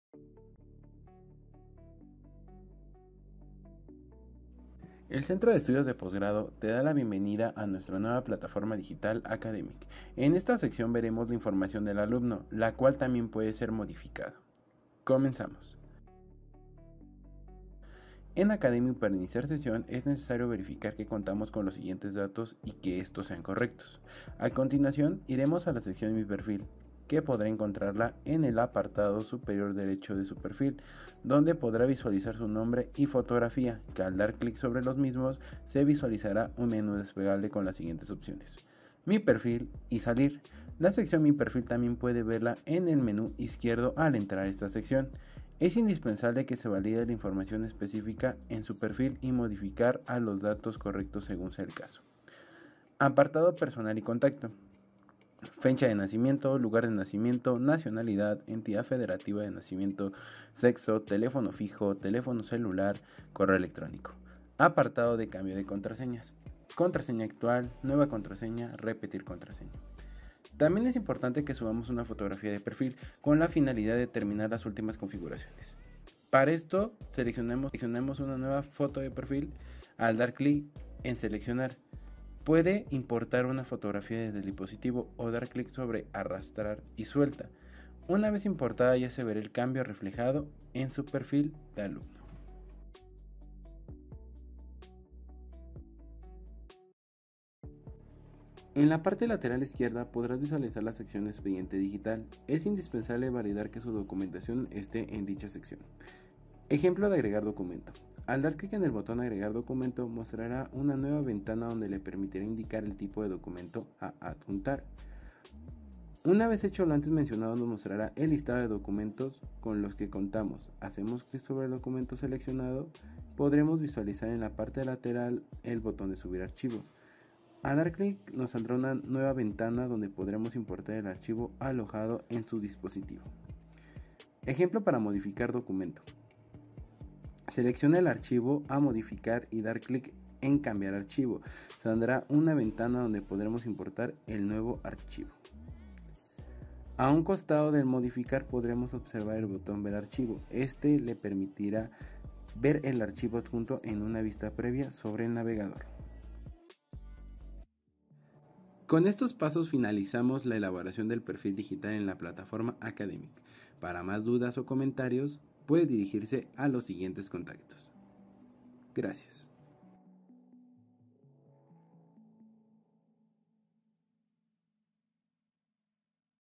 Perfil - Alumnos (Academic)
El Centro de Estudios de Posgrado te da la bienvenida a nuestra nueva plataforma digital (Academic).

En esta sección veremos la información del alumno, la cual también puede ser modificada. Para Iniciar sesión en Academic, es necesario verificar que contamos con los siguientes datos y que estos sean correctos.  ¡Comenzamos!